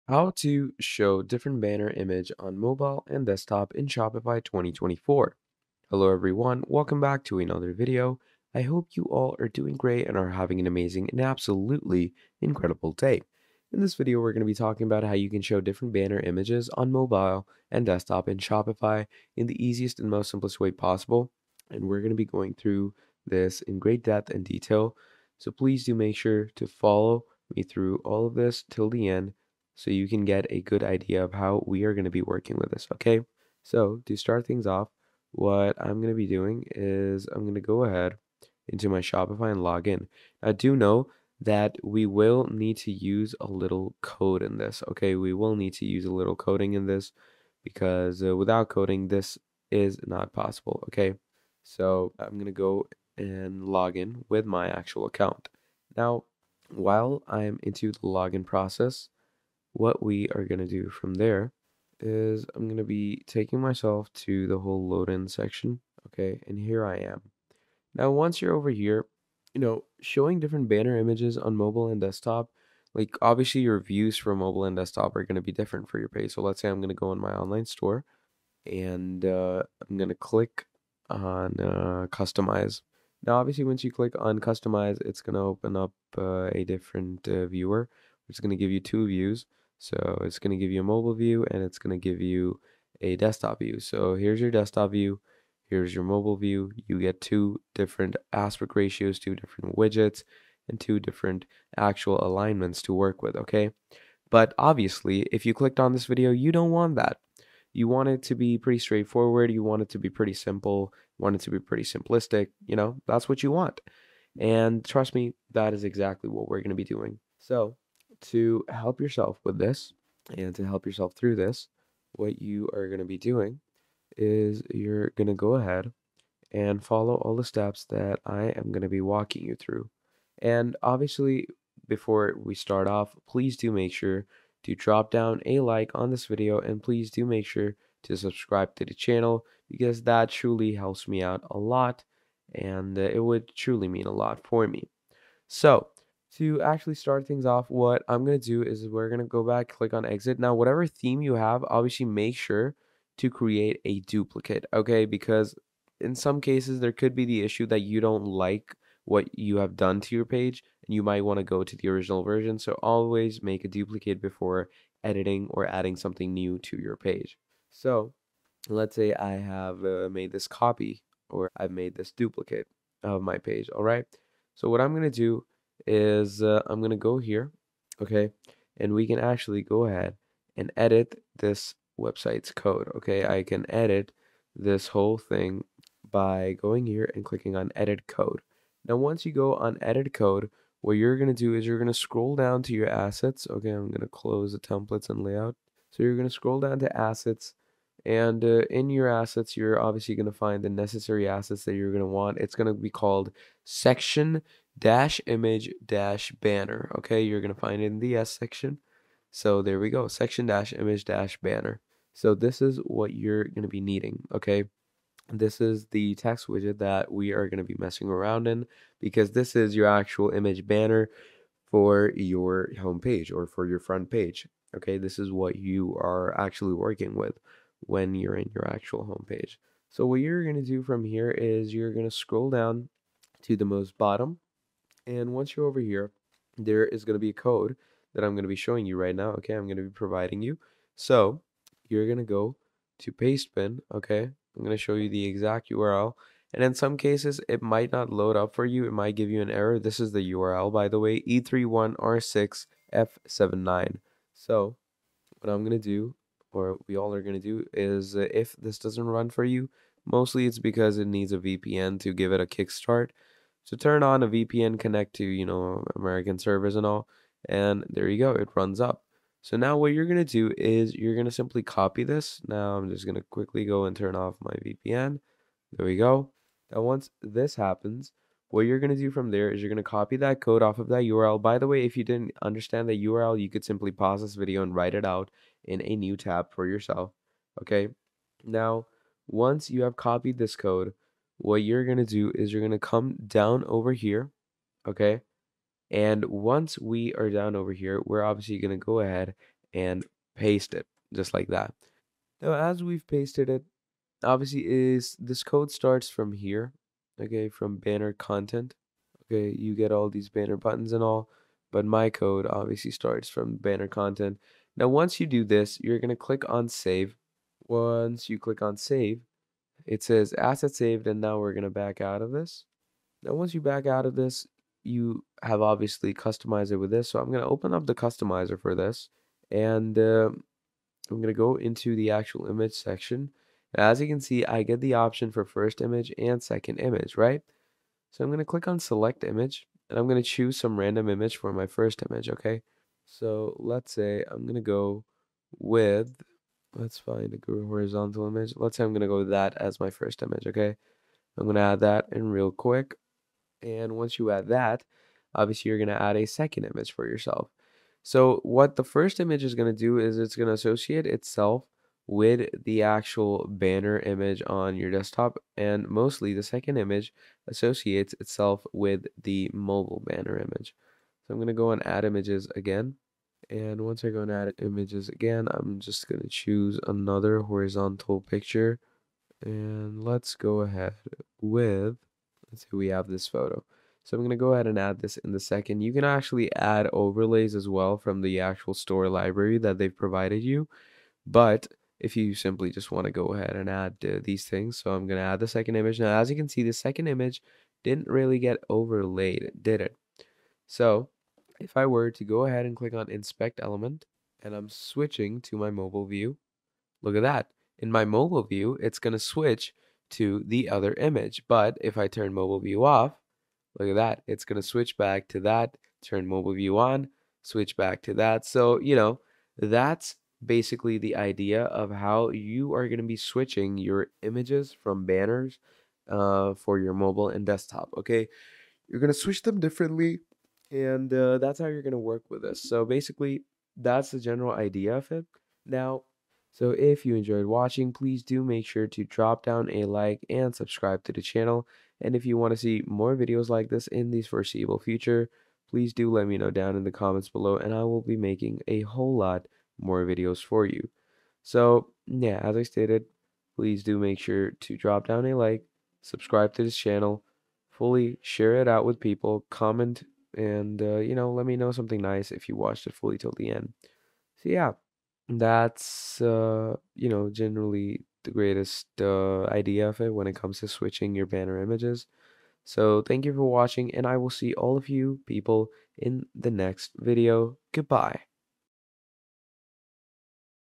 How To Show Different Banner Image On Mobile And Desktop In Shopify 2024
In this video we show you How To Show Different Banner Image On Mobile And Desktop In Shopify